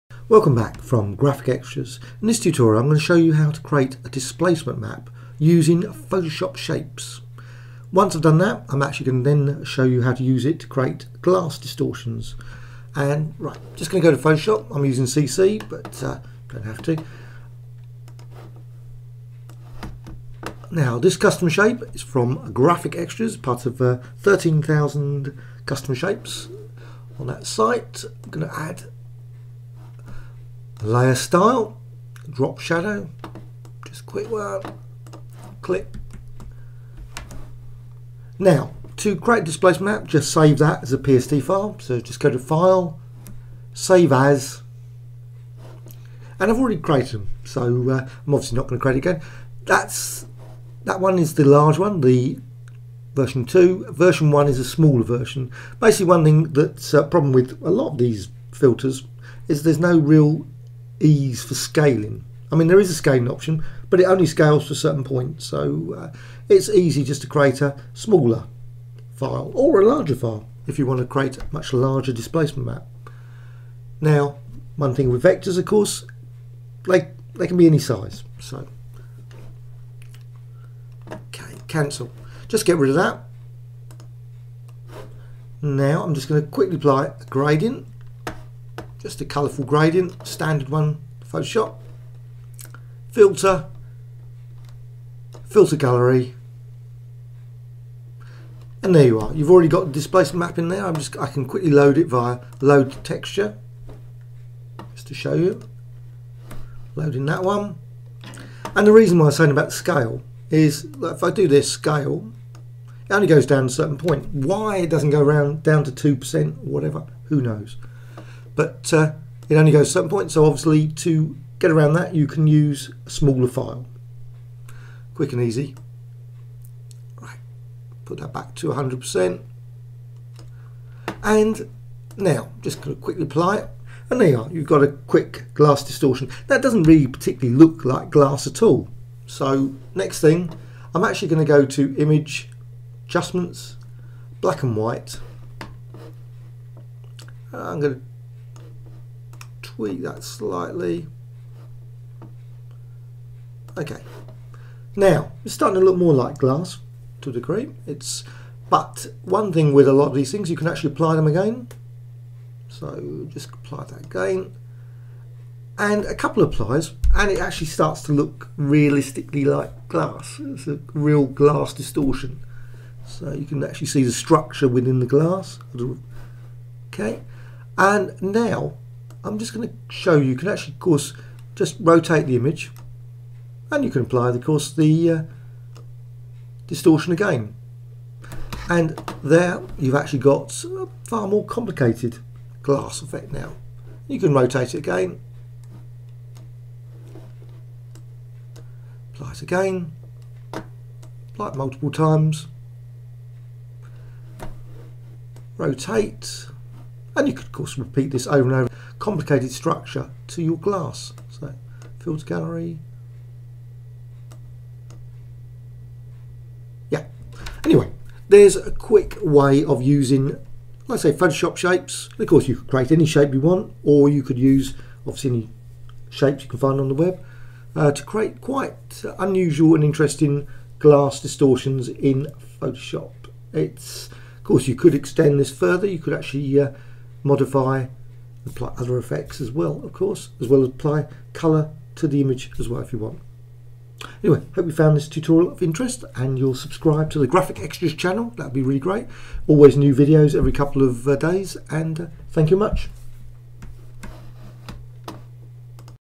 Photoshop shapes and displacement glass
A quick tutorial showing the creation of a displacement map using Photoshop shapes. The displacement map is then used as a source for a distortion using the Photoshop glass distortion filter. The filter is repeated and the image rotated and more displacements are added to create hi-impact glass effects